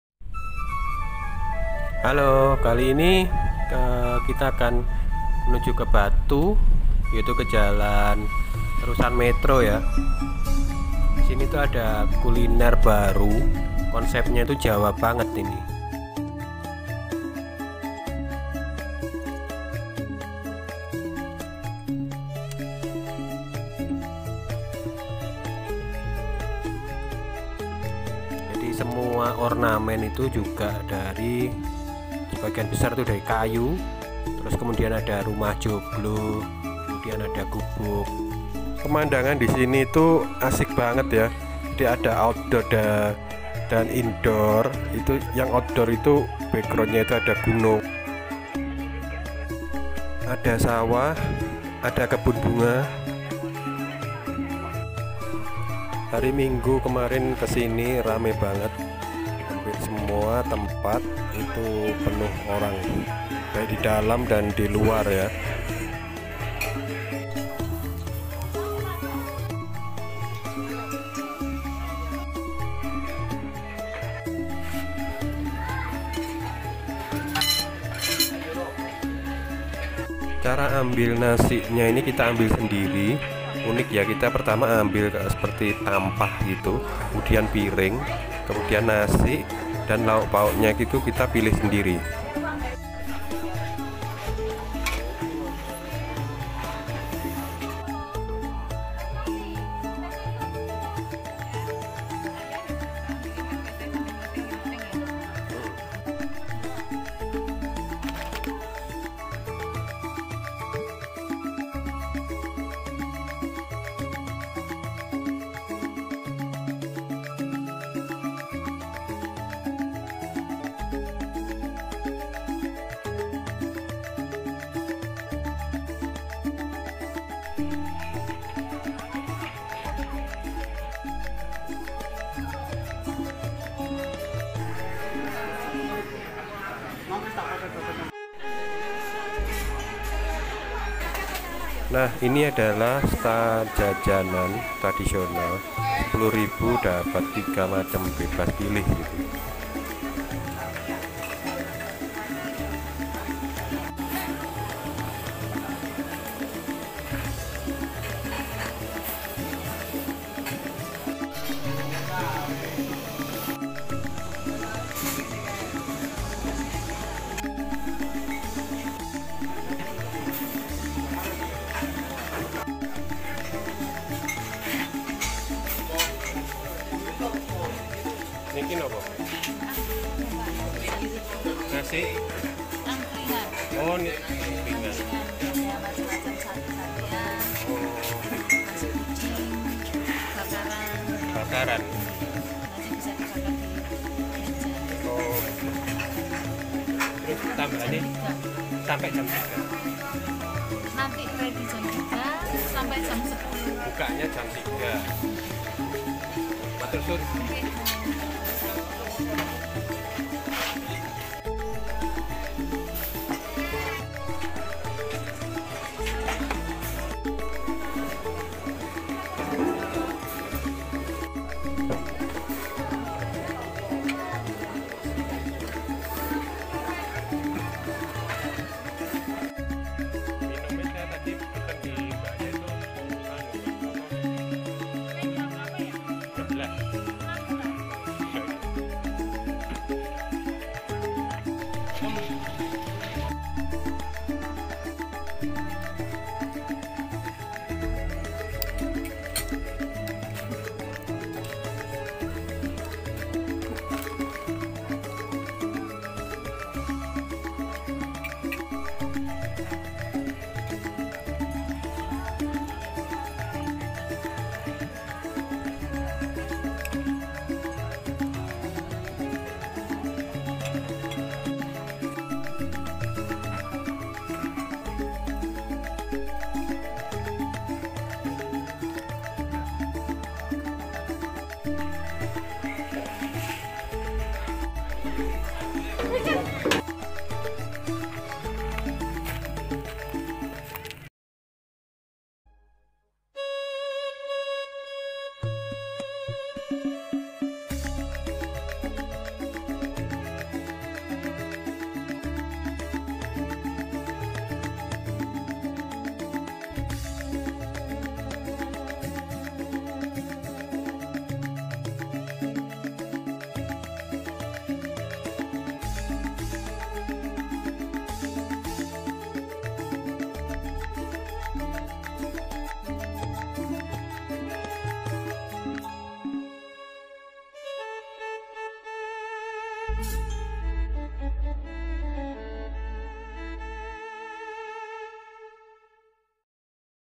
Omah Madhang Saridjoyo Rame Banget Serba Antri
Kuliner baru Kota Batu, konsep Jawa , view pemandangan seger banget